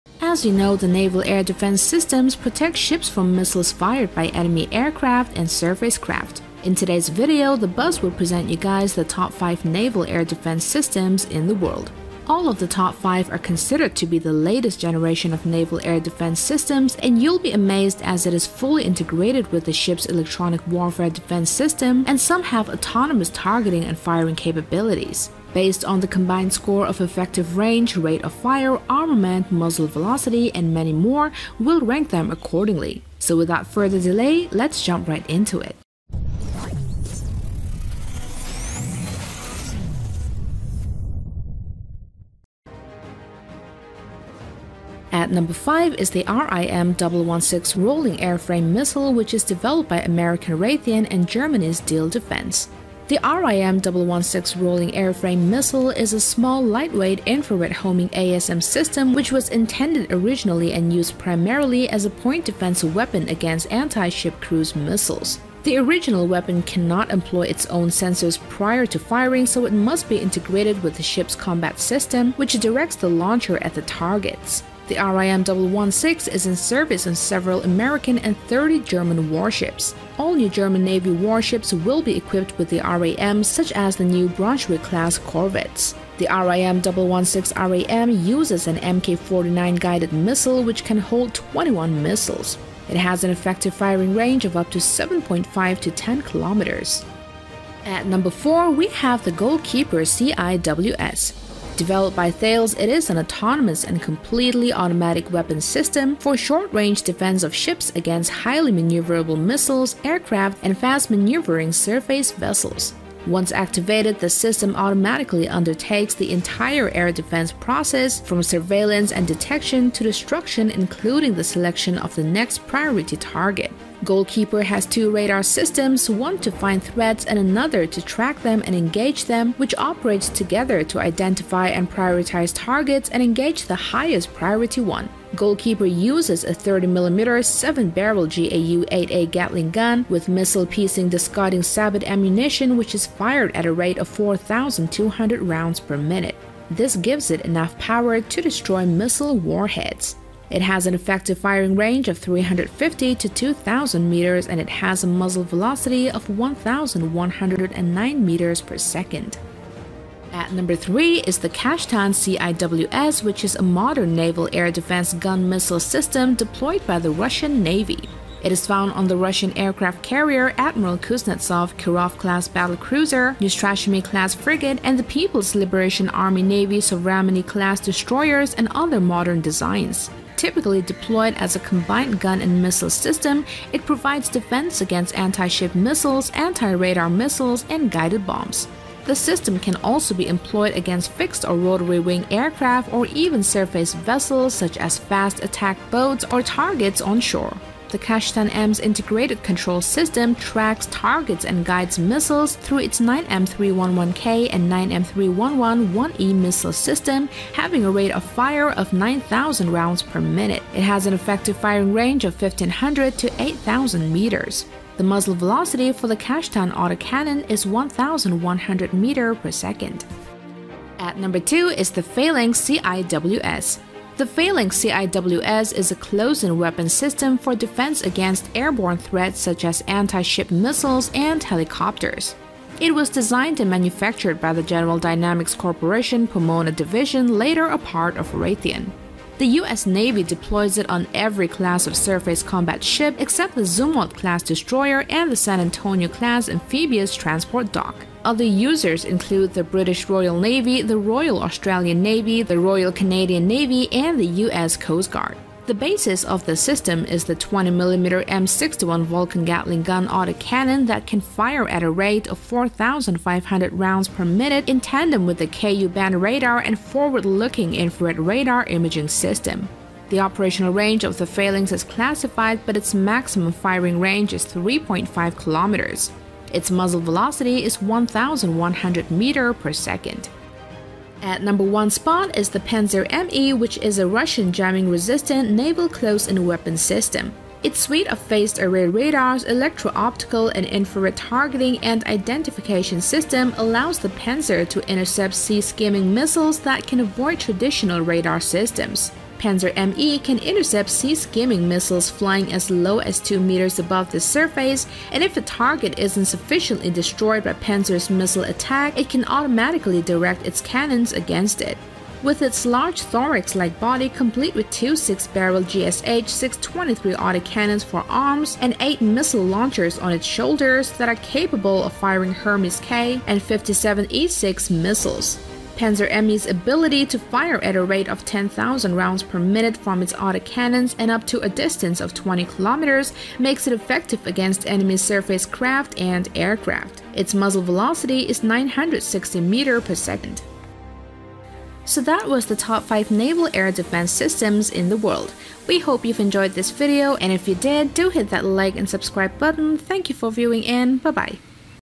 Top Naval Air Defence Systems | Most Powerful Naval ADS in the World (2020)
Video Information: Naval air defense systems protect ships from missiles fired by enemy aircraft and surface craft. In today’s video, the Buzz will present you guys the top 5 naval air defense systems in the world. All of the top 5 are considered to be the latest generation of naval air defense systems and you’ll be amazed as it is fully integrated with a ship’s electronic warfare defense system and some have autonomous targeting and firing capabilities. Based on the combined score of effective range, rate of fire, armament, muzzle velocity and many more, we’ll rank them accordingly. Enjoy watching. Cheers!

The Buzz does not own the rights to these pictures.